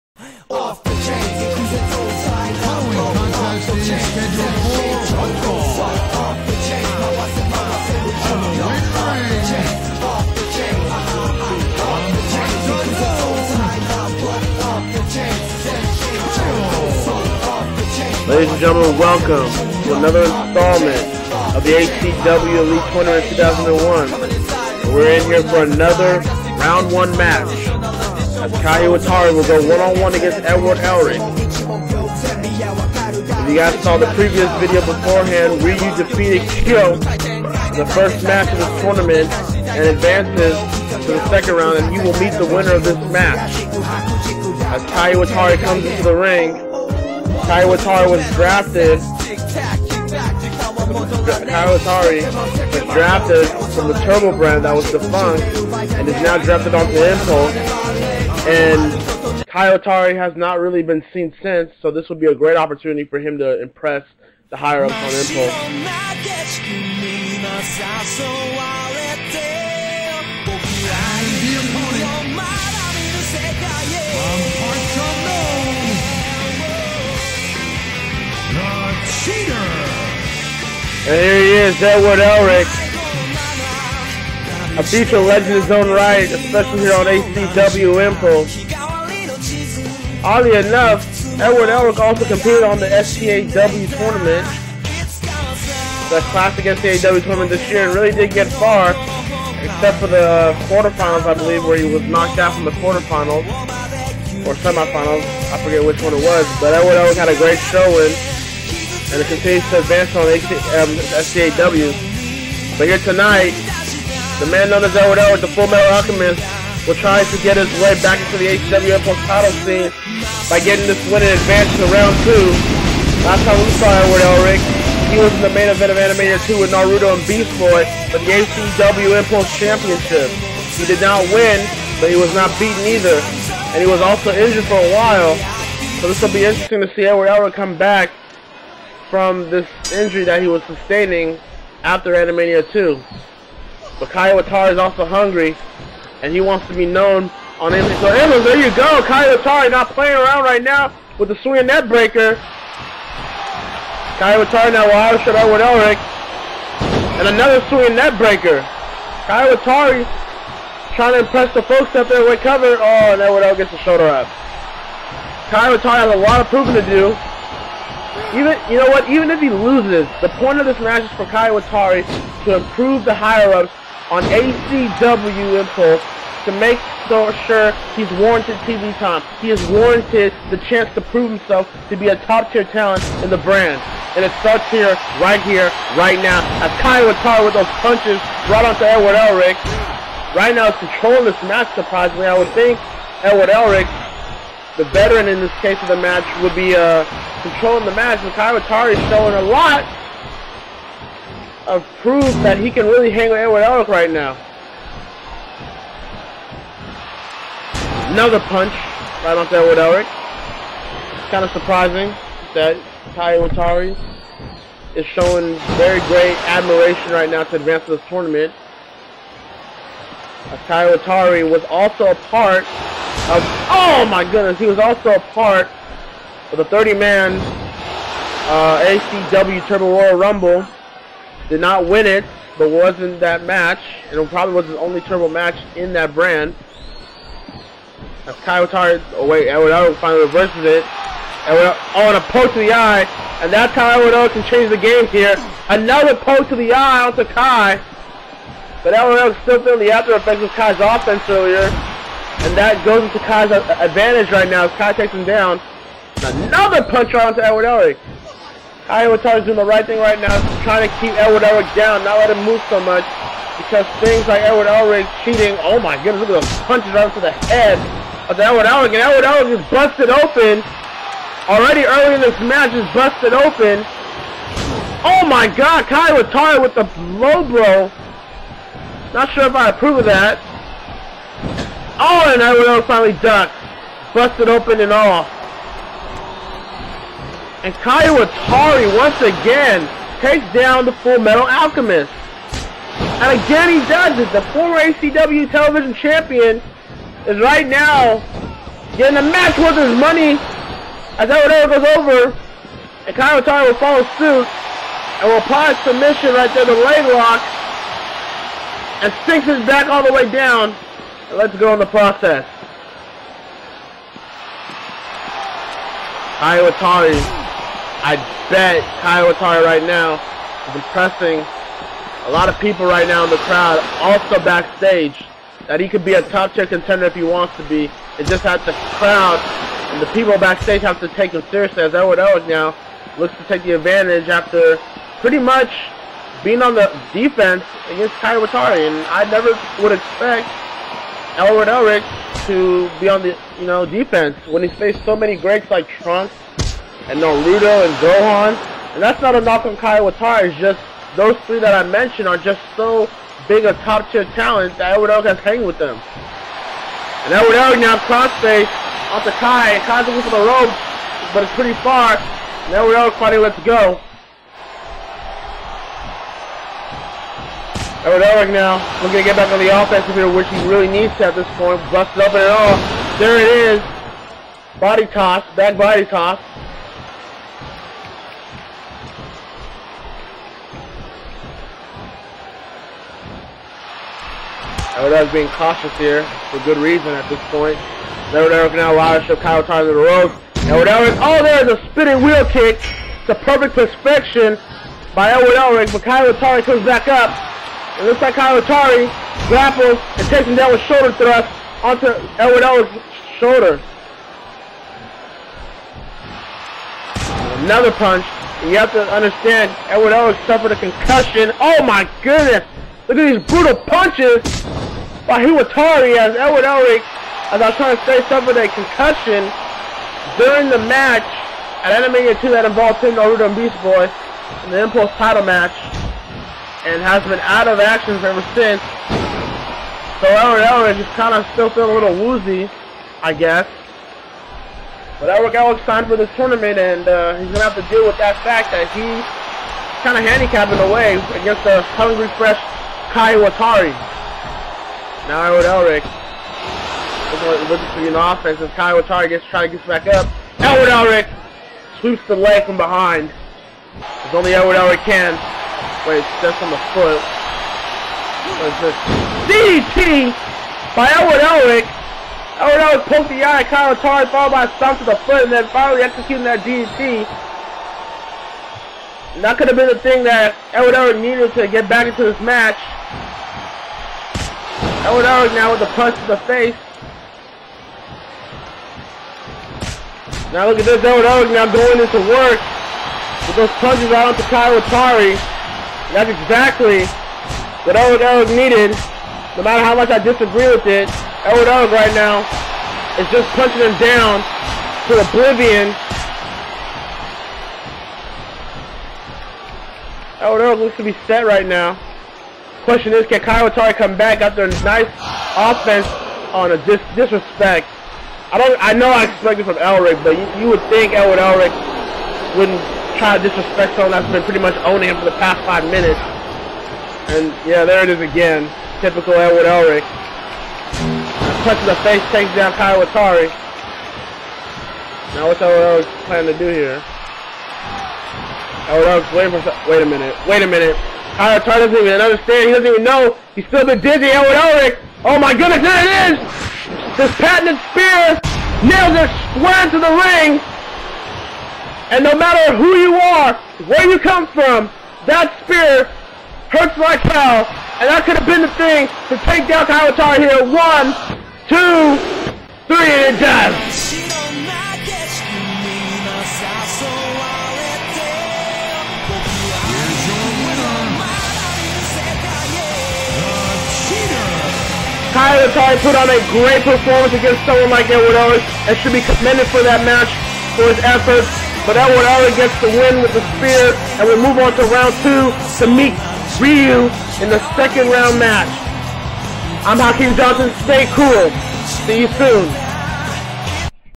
2011 ACW Elite Tournament - Round 1 - Match 2 - Kai Hiwatari v. Edward Elric
The tournament continues as Kai Hiwatari takes on Edward Elric.